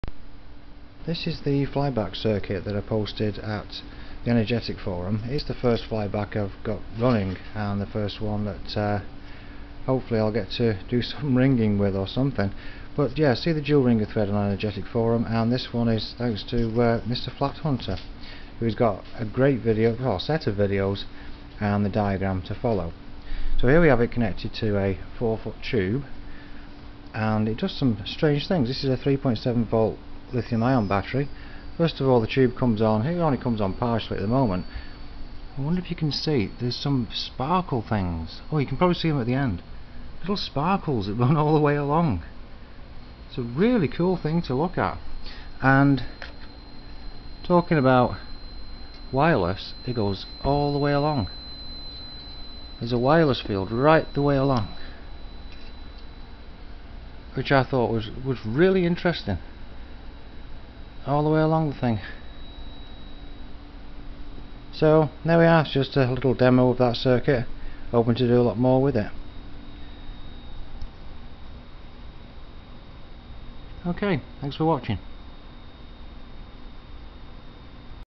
3.7V flyback - tube sparkles and 4ft of wireless
A quick demo of the first flyback to run (from 5 tried). Hoping to do more with this circuit and catch up to the folks who are making Joule Ringers from such ideas.

See MrFlathunter's channel for the circuit.

Transistor C5198
100ohm resistor in series with 20K tuning pot
Salvaged Samsung FCK 4182 flyback

Note - don't attempt unless proper safety precautions are taken. These flyback transformers can produce deadly voltages !